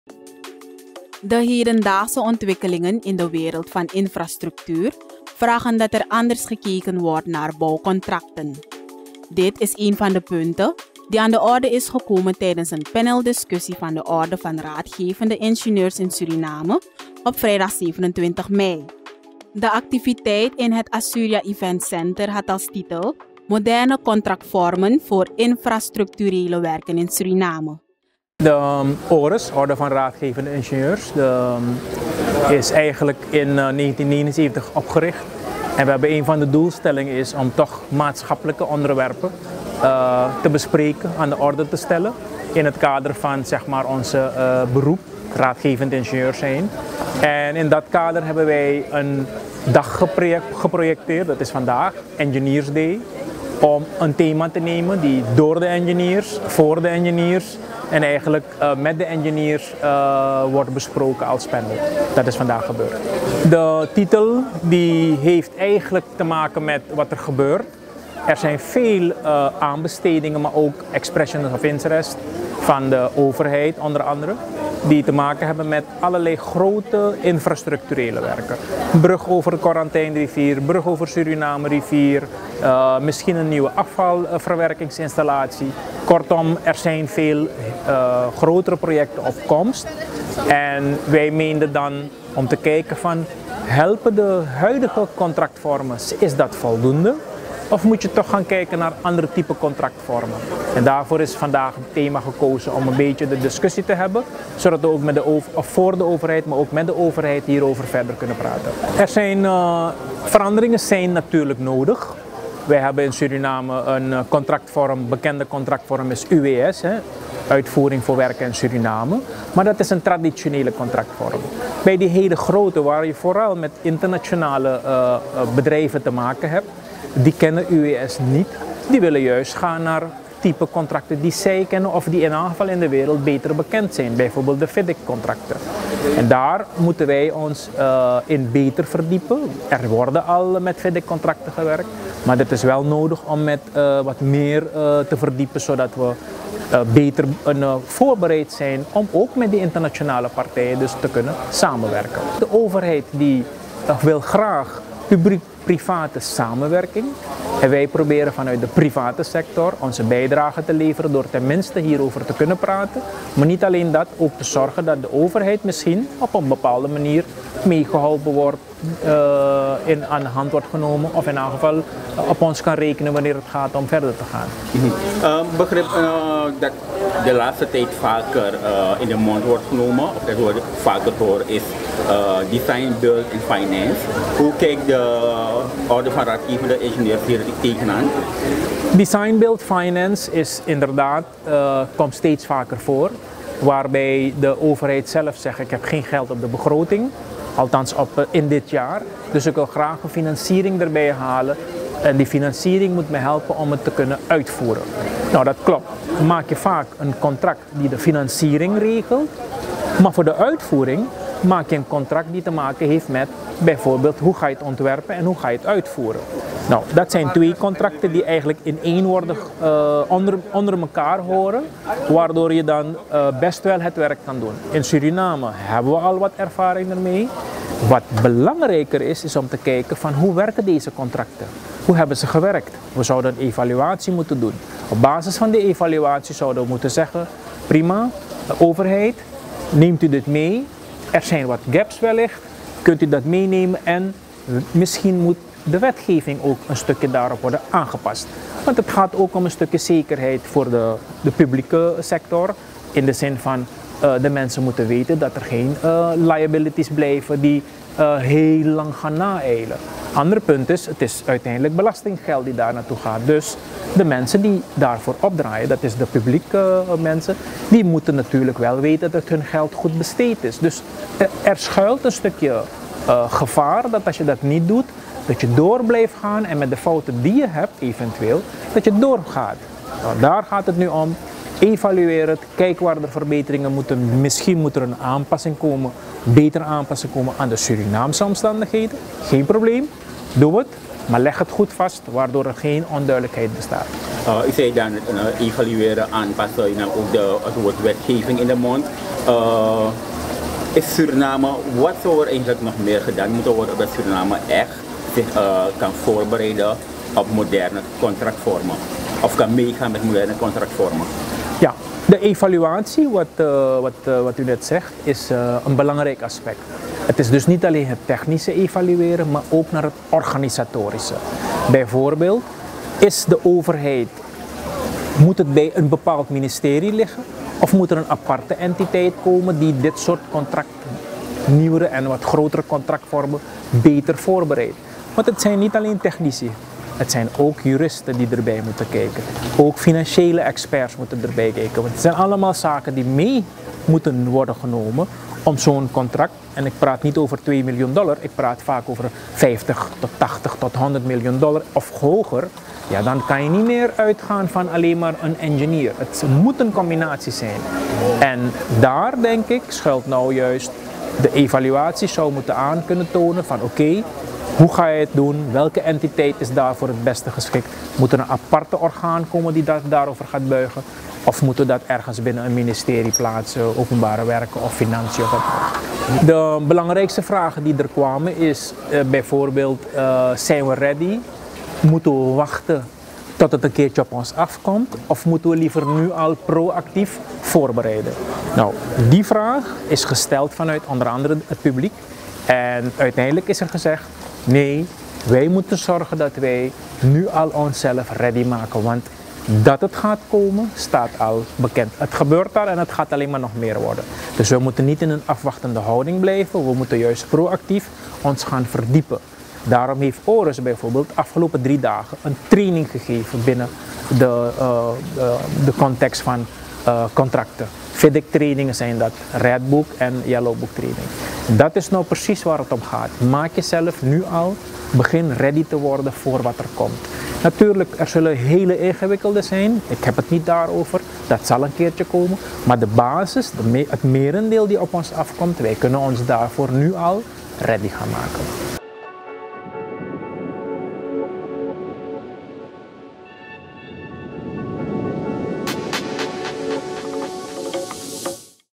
ORDE VAN RAADGEVENDE INGENIEURS BUIGT ZICH OVER MODERNE CONTACTVORMEN EN INFRASTRUCTURELE WERKEN
De hedendaagse ontwikkelingen in de wereld van infrastructuur vragen dat er anders gekeken wordt naar bouwcontracten. Dit is een van de punten die aan de orde is gekomen tijdens een paneldiscussie van de Orde van Raadgevende Ingenieurs in Suriname (ORIS) op vrijdag 27 mei 2022. De activiteit in het Assuria Event Center had als titel: "Moderne contractvormen voor infrastructurele werken in Suriname". Wij spraken ORIS-voorzitter ingenieur Ravindra Patandin.